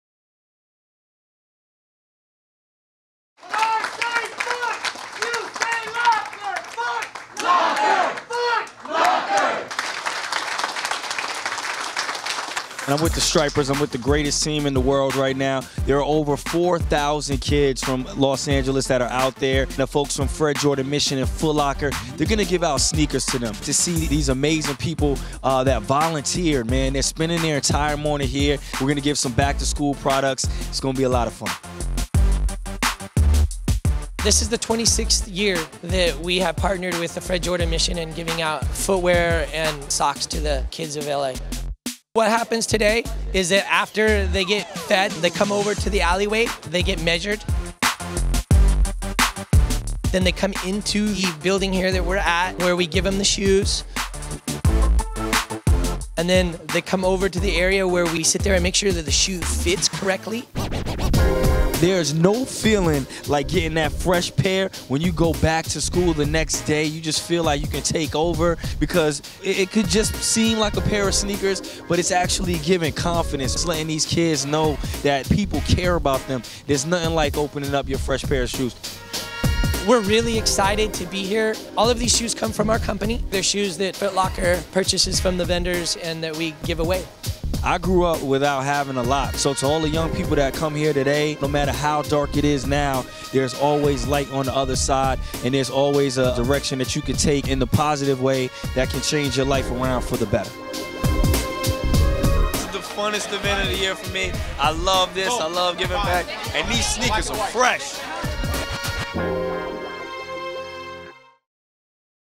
Fred Jordan Mission & Foot Locker Give Away Shoes to Kids in Need
Fred Jordan Mission and Foot Locker teamed up to give over 4,000 underprivileged children shoes as they got started on the new school year. Terrence 'J' Jenkins, co-anchor of the news program E! and a former Foot Locker employee helped host the event that brought smiles to thousands of kids.